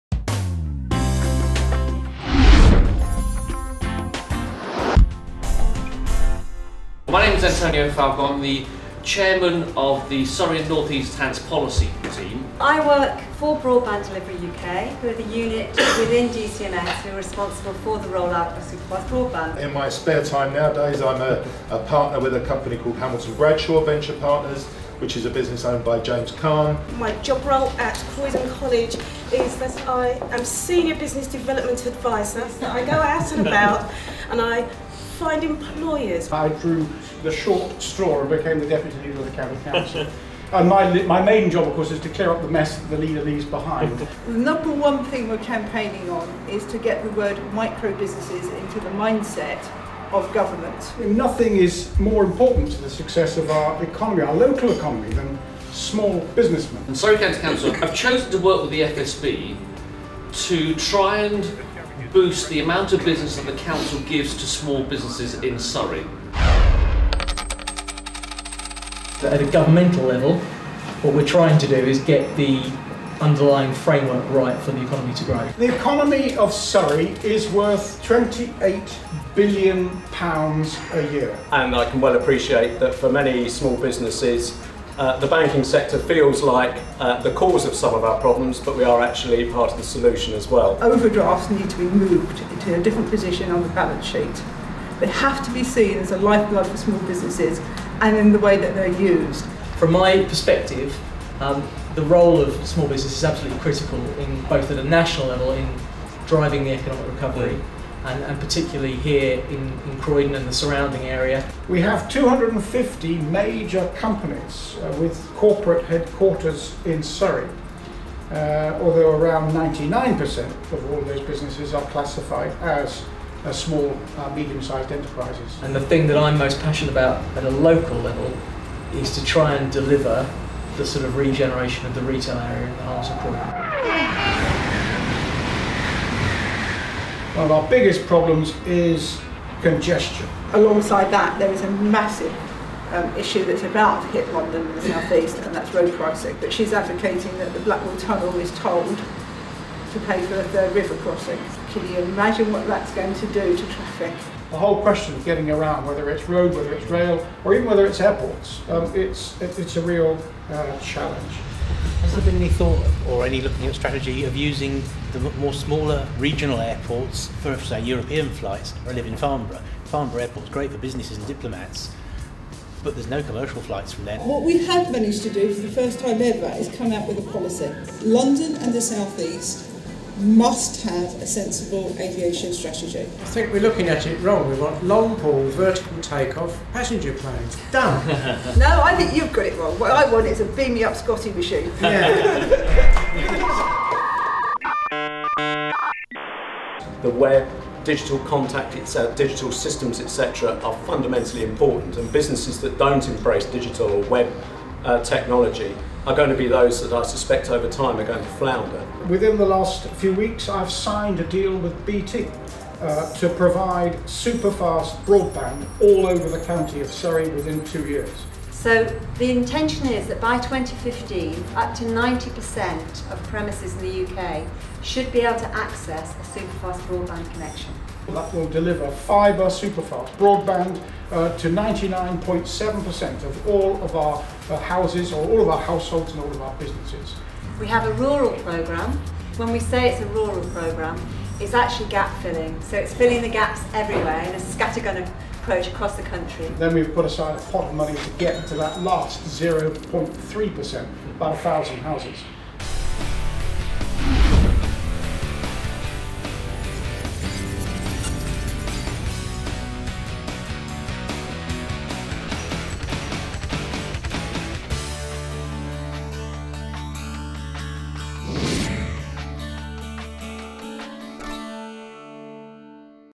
FSB Croydon Policy Conference
Federation of Small Businesses Surrey Policy Team working to improve members businesses